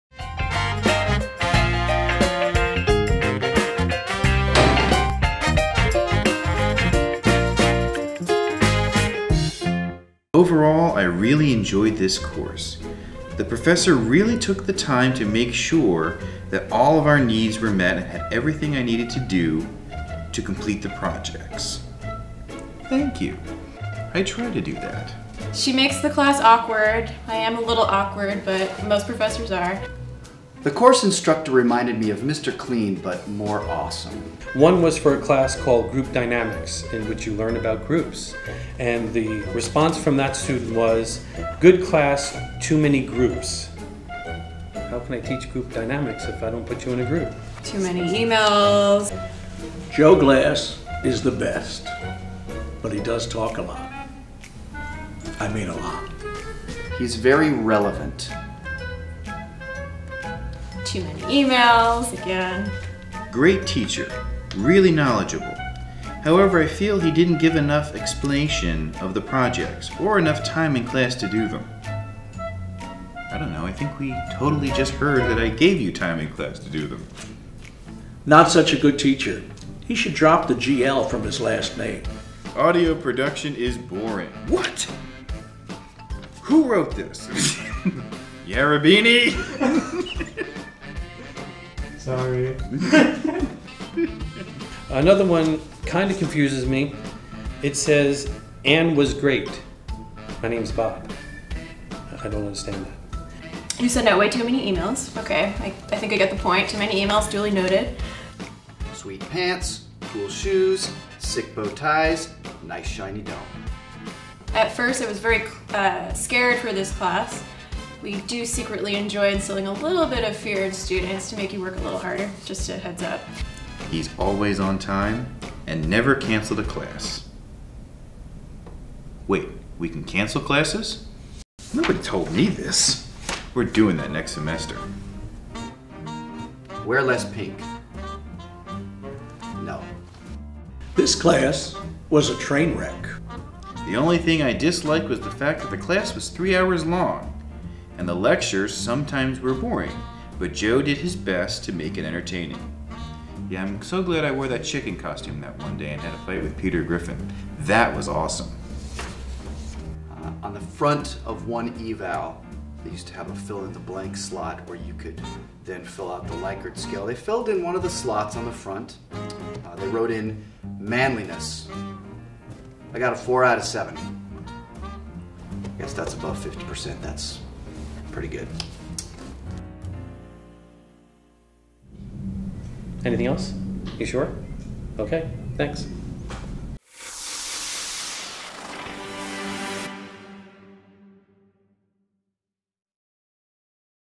Professors Reading Their Course Evaluations
Everyone knows the semester is ending when the course evaluations begin. Some of our professors share their favorite, funniest, and sometimes plain weird evaluations.

Thanks to Dr. Glass, Dr. Johnson, Dr. Kennedy, Dr. Montalbano, and Joe. Bonadonna for their time!

Cinematography - Andrew Pham, Mike Yarabinee, Erin Martz, Sean McDonald

Post Production - Andrew Pham, Sean McDonald